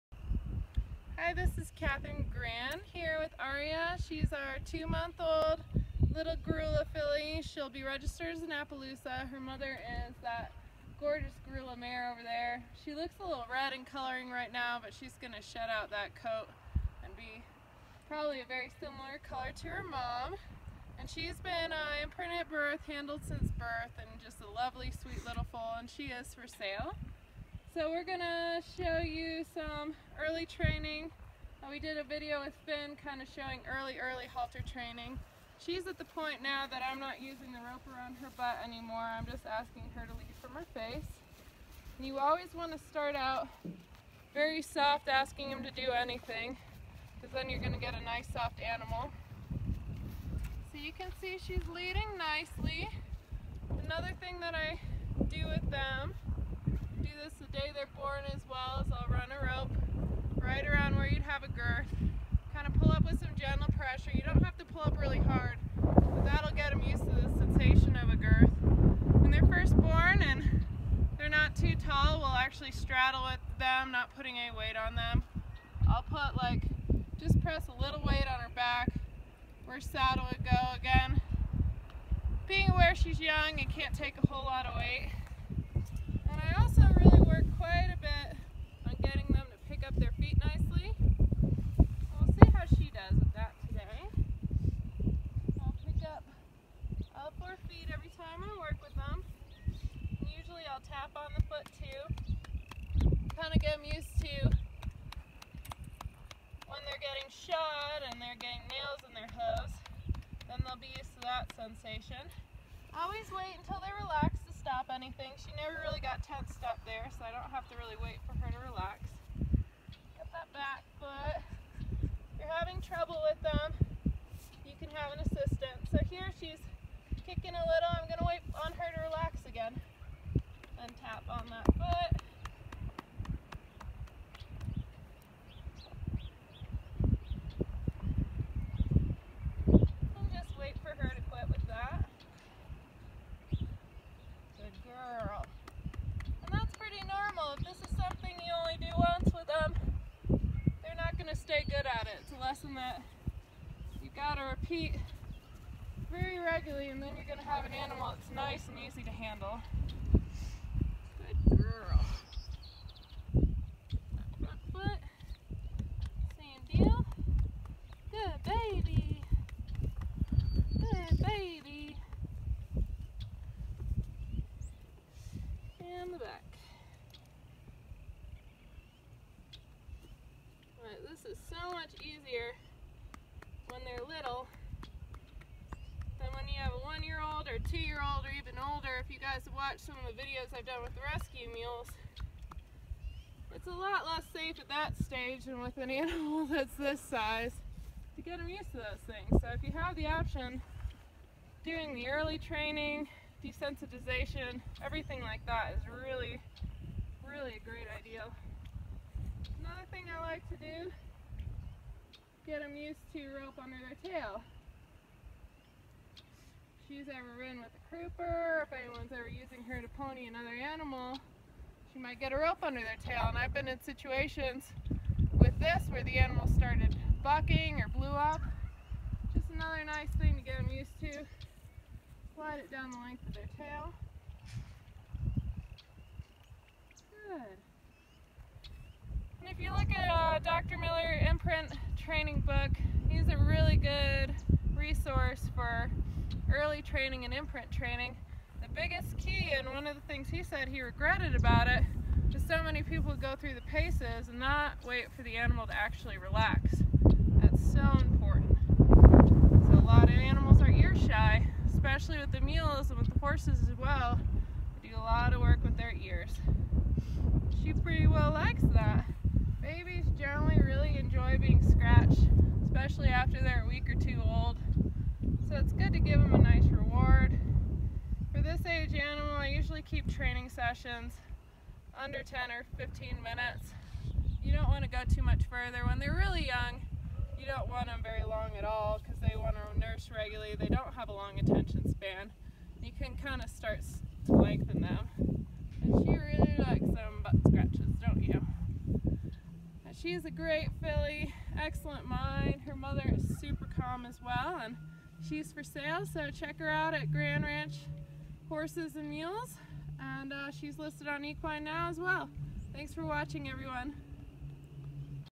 Halter Training Aria
Halter training our gorgeous grulla filly Aria.  Aria is for sale!